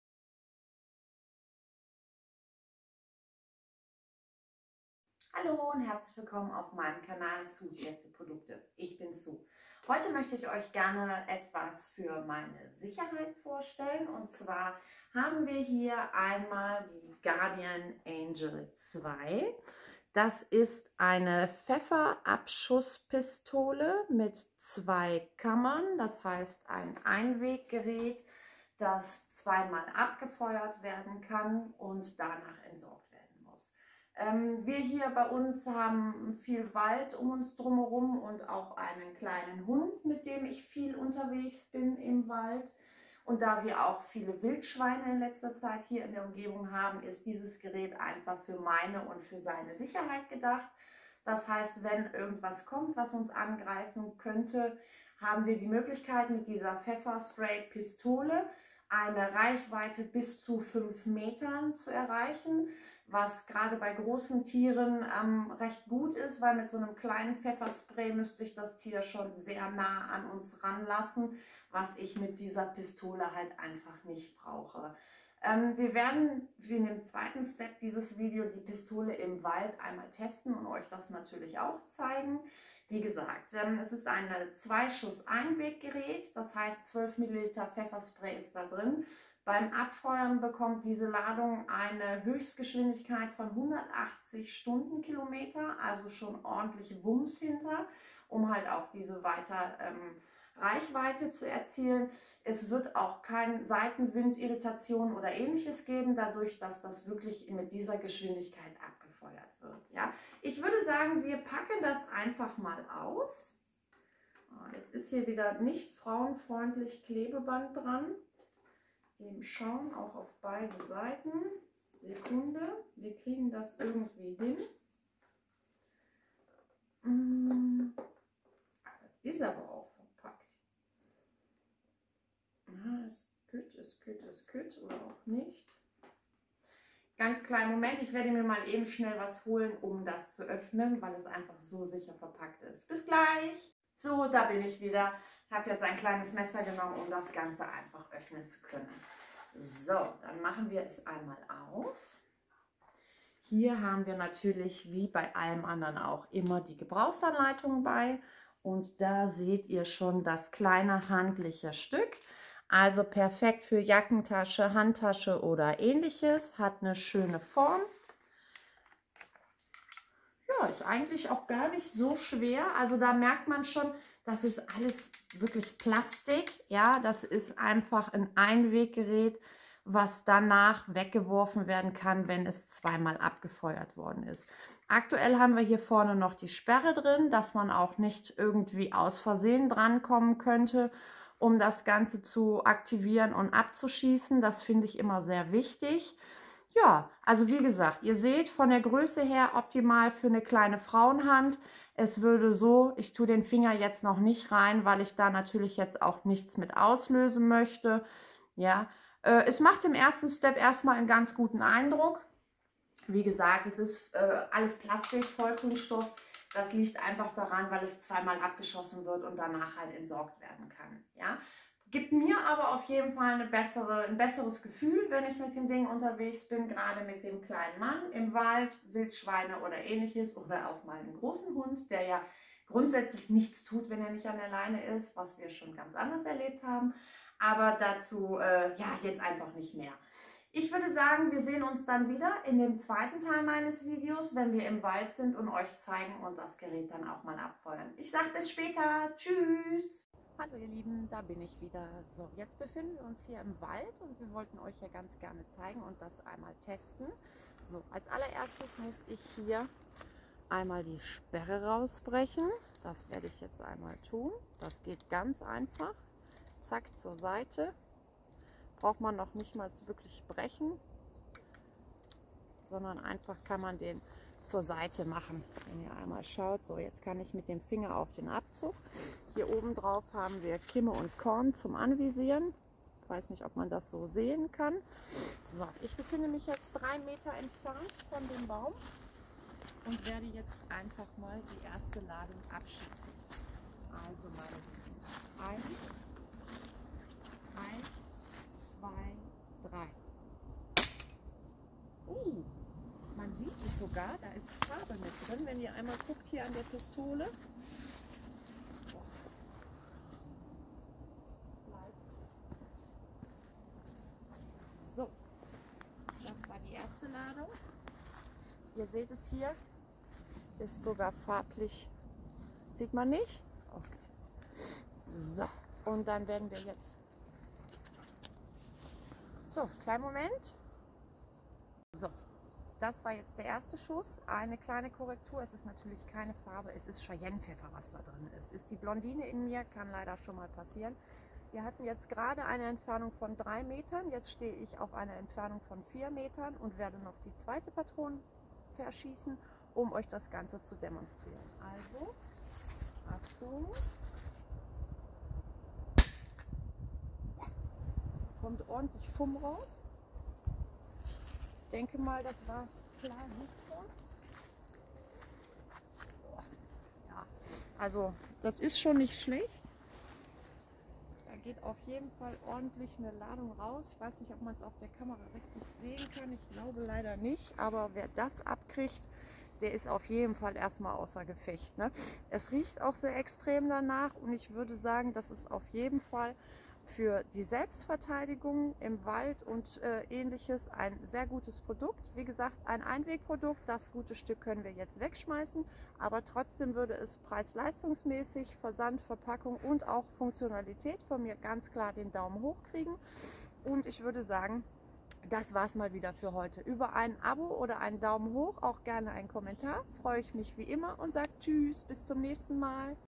Piexon Pfefferspray Abschussgerät Guardian Angel II,
Su testet für euch:

Piexon Pfefferspray Abschussgerät Guardian Angel II, Anthrazit, 130148

WIR HABEN NACH UNSEREN SCHIEßÜBUNGEN DIE PFEFFERRÜCKSTÄNDE IM WALD ENTFERNT UM KEINE TIERE ZU VERLETZEN !

Instagramm :  su_und_mimi